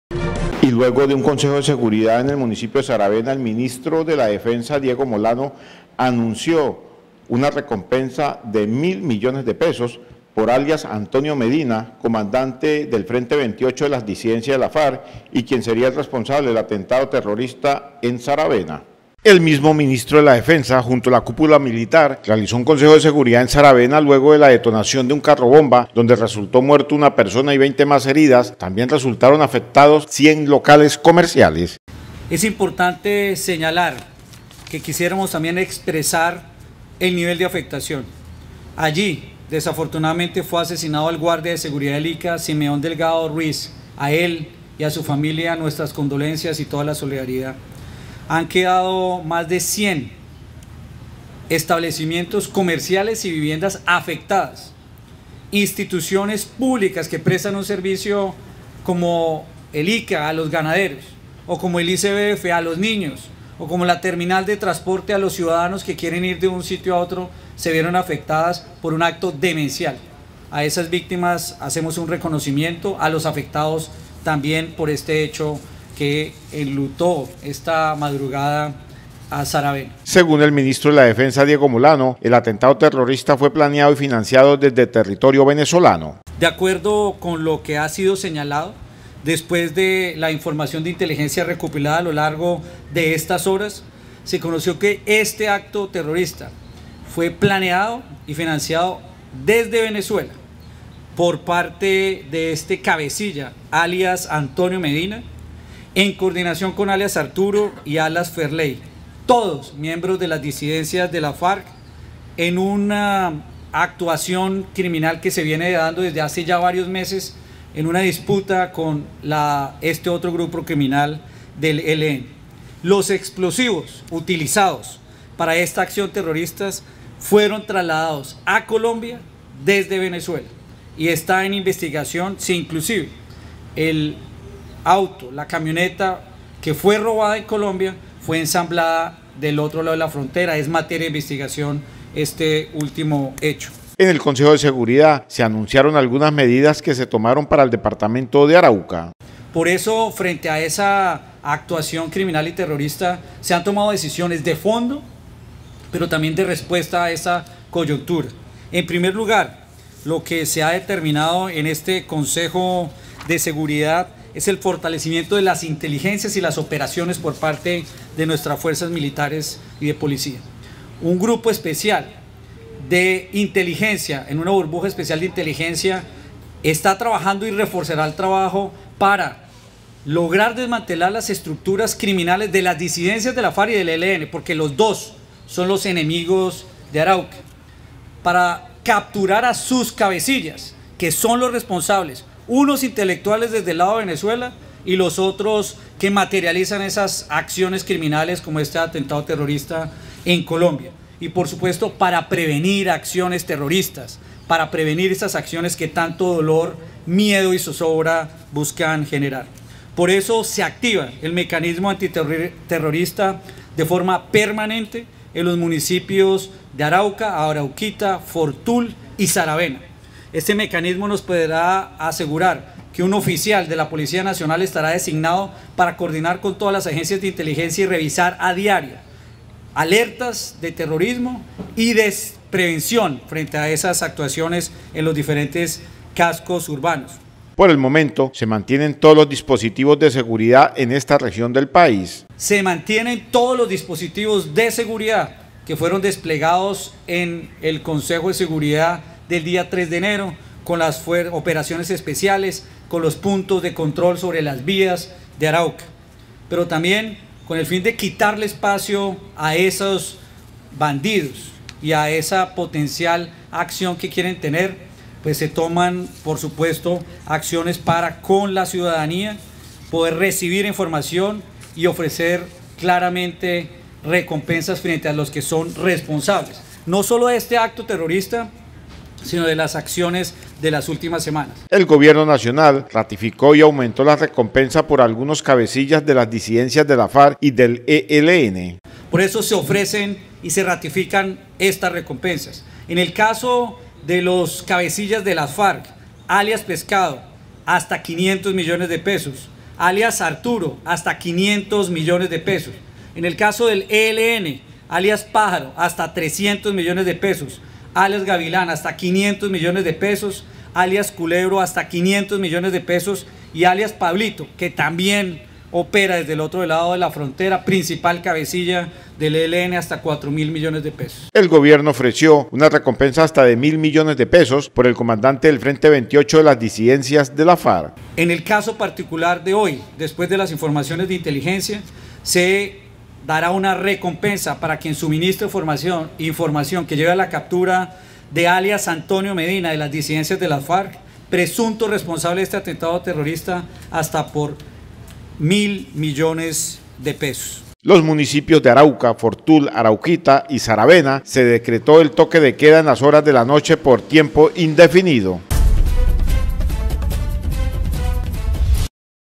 Ofrecen Mil Millones de Pesos de Recompensas por alias "Antonio Medina" Responsable del Atentado
Ofrecen Mil Millones de Pesos de Recompensas por alias "Antonio Medina" Responsable del Atentado en Saravena